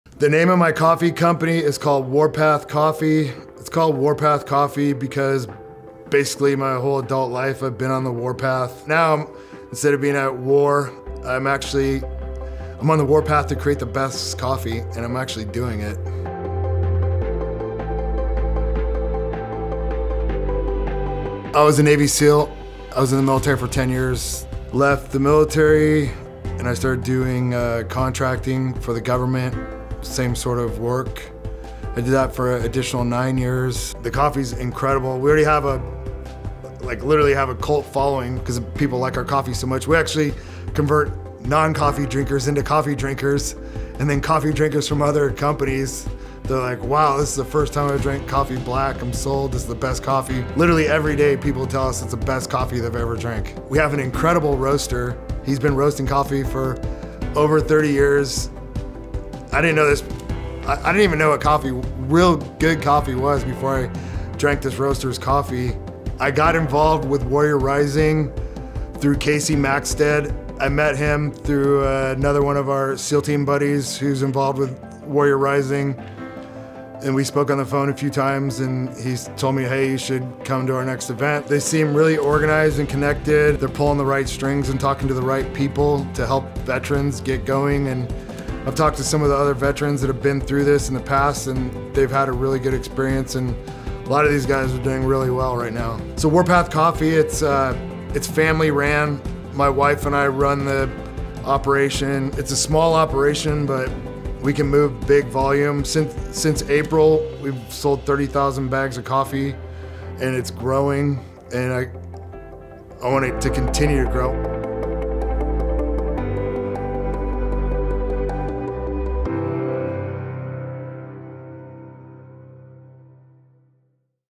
Tej Gill & Warpath Coffee - 2022 San Antonio Business Shower Cohort Member
Former Navy Seal, Government Contractor, and Founder of Warpath Coffee - Tej Gill met a member of the Warrior Rising staff and knew he wanted to got through the Warrior Academy.

Tej runs the operation with his wife, and they are on the Warpath to convert non-coffee drinkers, into coffee lovers!

Diesel Jack Media is the official Marketing and Video Production Agency of Warrior Rising, check them out and let them know we sent you!